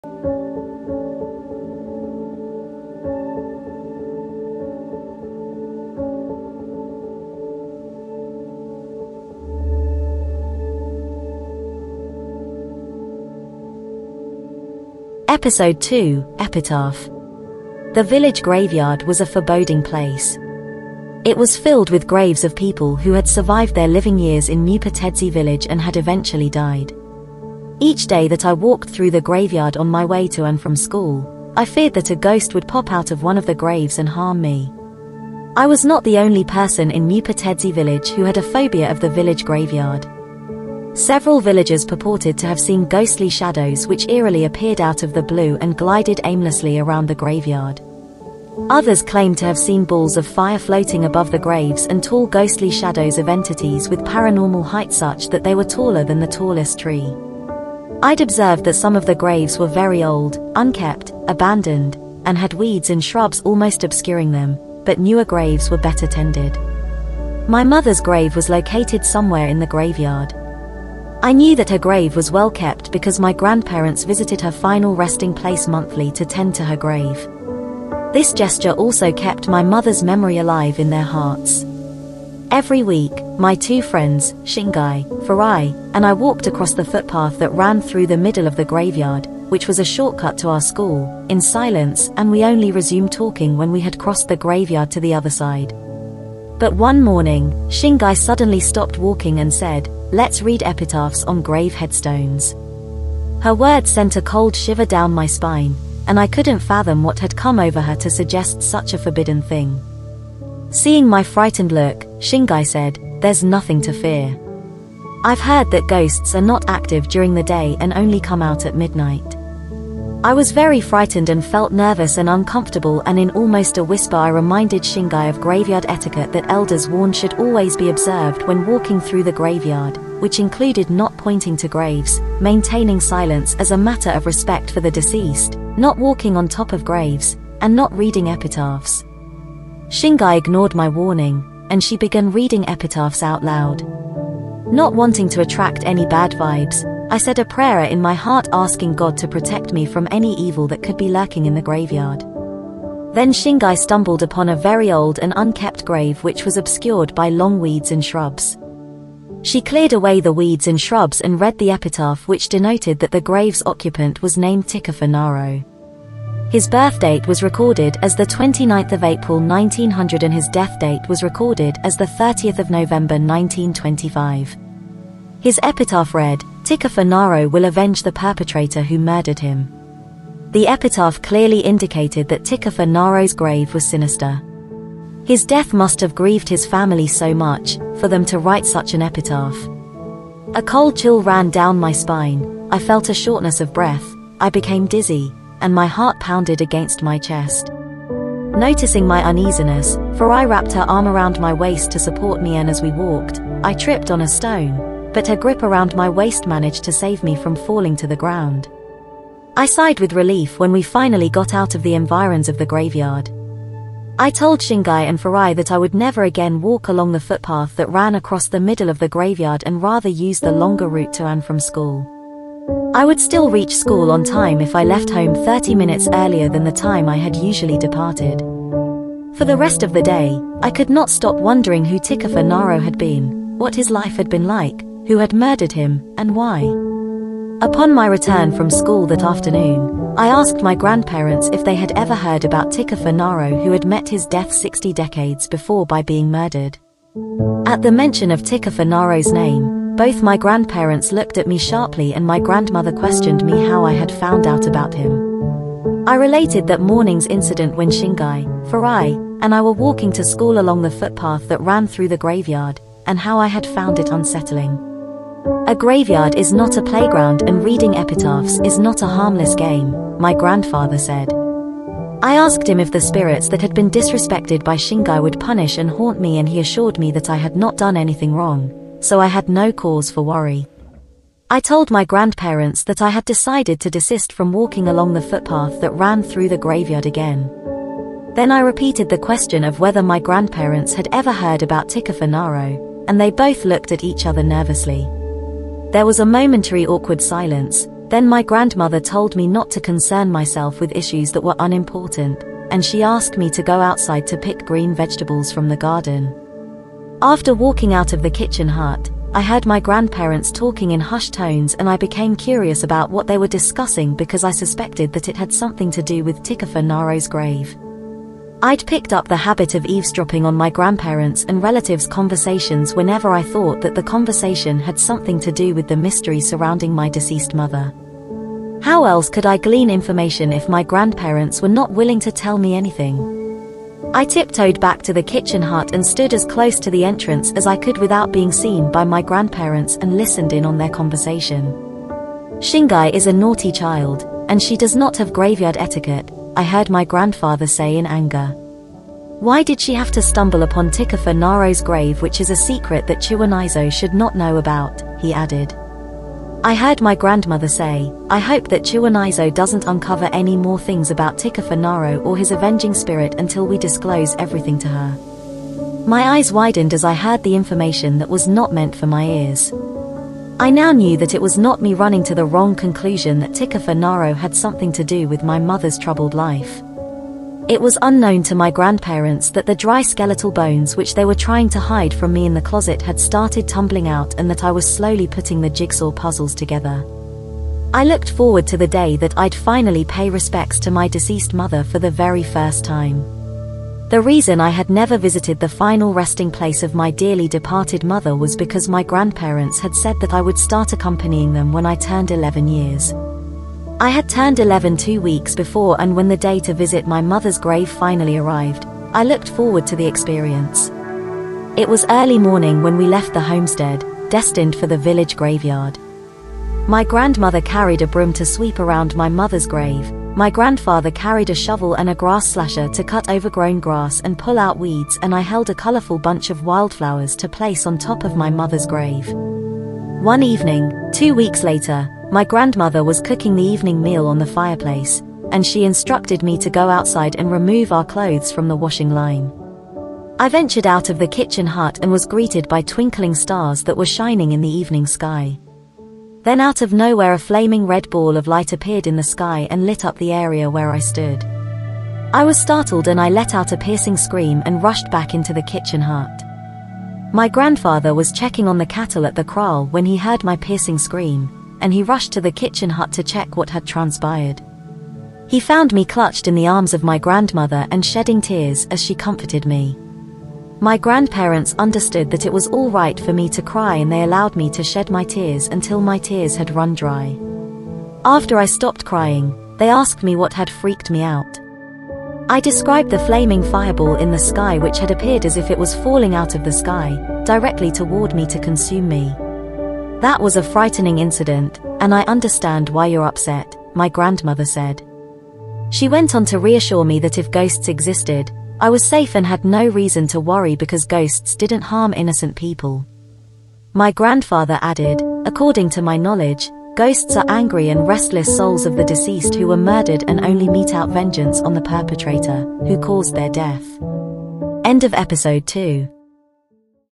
A WOMAN'S WORST NIGHTMARE (EPISODE TWO) - EPITAPH
Due to superstitious beliefs regarding graveyards, Chiwoniso feared the village graveyard. But every week she and her friends braved walking across the footpath that ran through the middle of the graveyard, on their way to and from school because the footpath that ran across the middle of the graveyard was a shortcut to their school.